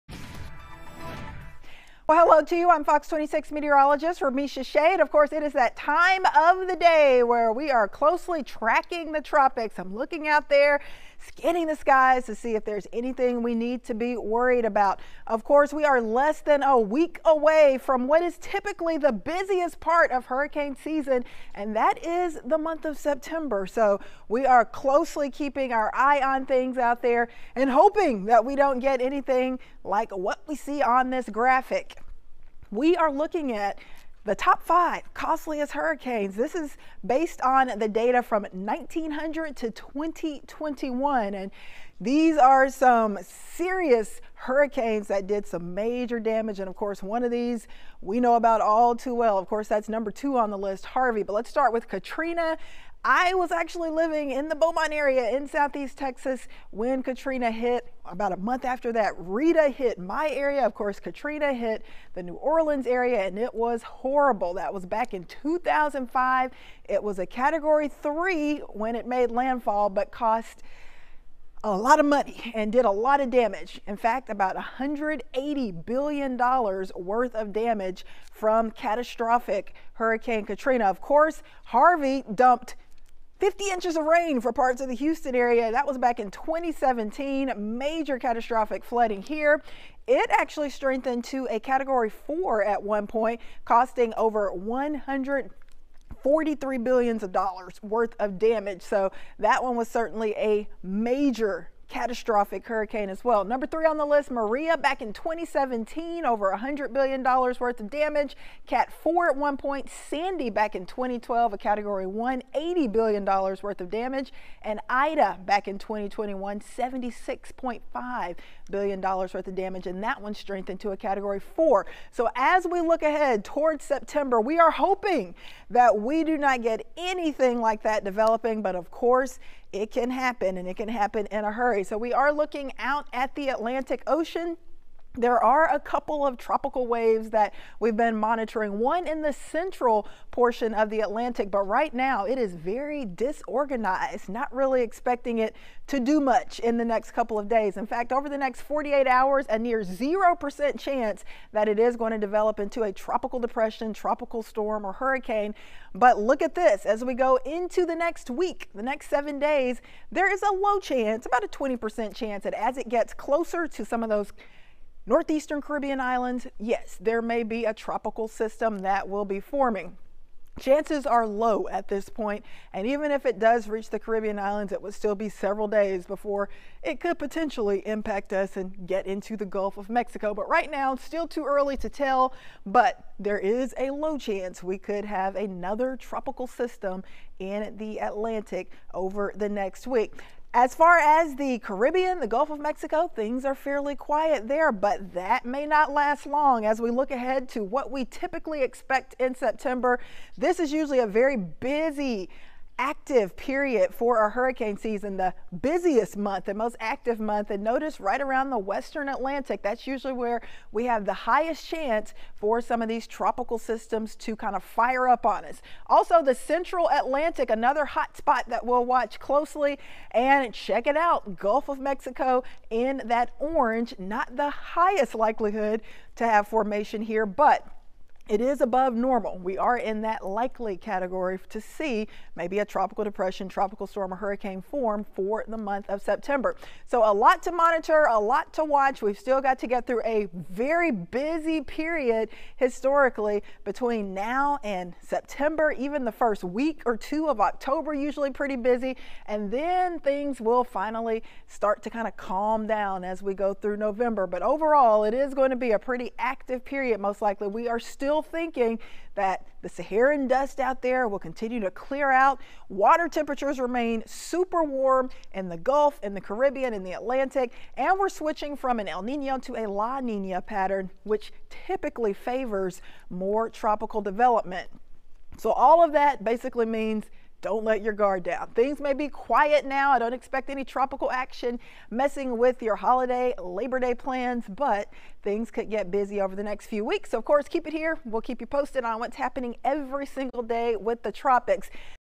Area in Atlantic being watched for possible development | Tropical Weather Forecast
An area of showers and storms in the Central Atlantic is still being monitored for possible tropical development. Development, if any will be slow with a low 20% chance to turn into our next tropical depression, storm or hurricane over the next week. This could happen by the middle of next week near the Lesser Antilles. The next name up would be Francine. Very small and weak disturbance southeast of Bermuda now is down to a 0% chance for tropical development.